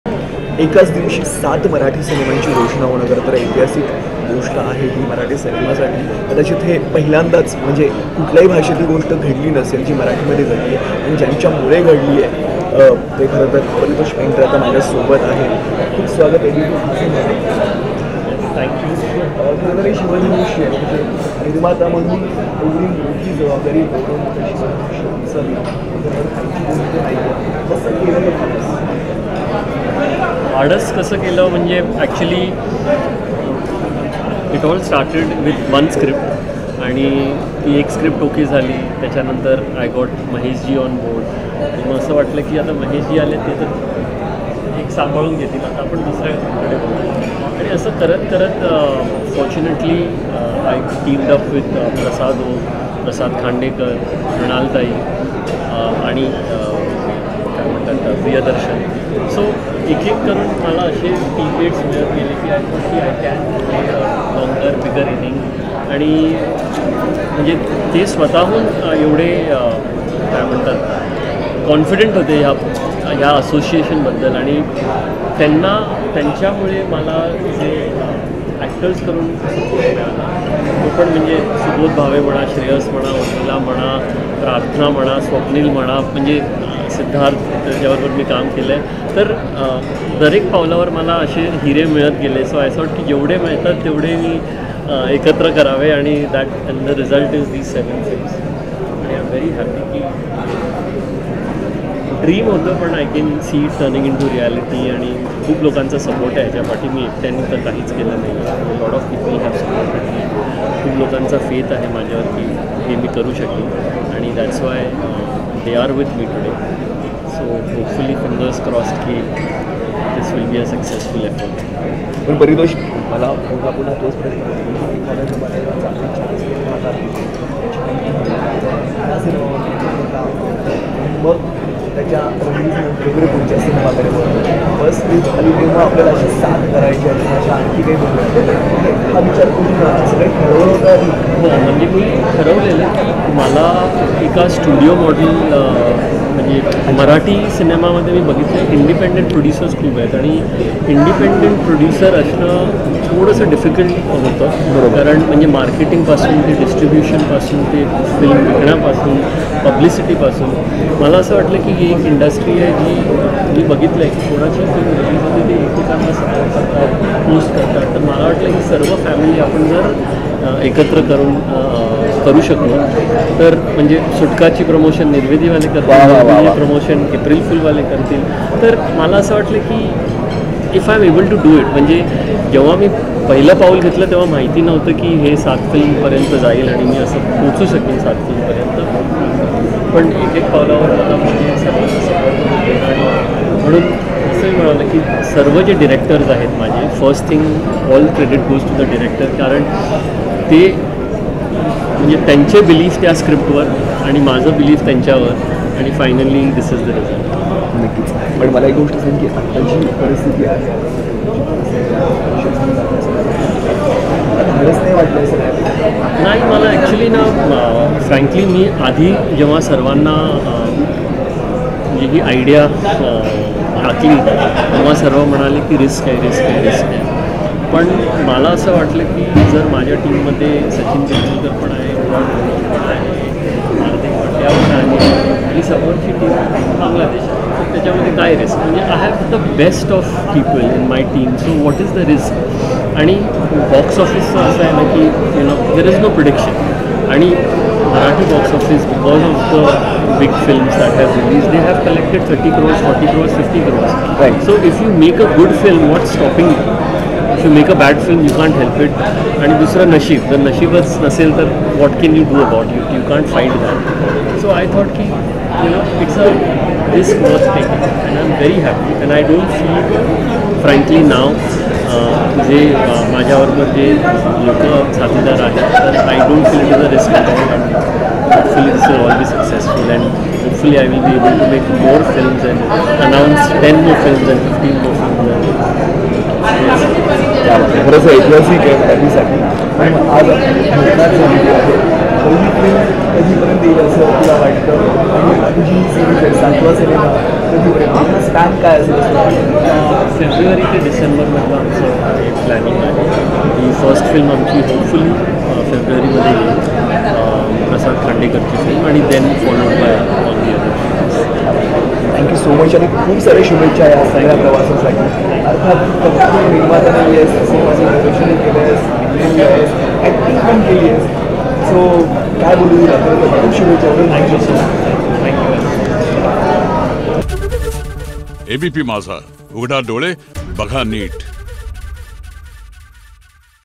Paritosh Painter 7 Marathi Films : 7 मराठी चित्रपट एकत्र लाँच करण्यामागची कल्पना नेमकी काय?
Paritosh Painter : 7 मराठी चित्रपट एकत्र लाँच करण्यामागची कल्पना नेमकी काय?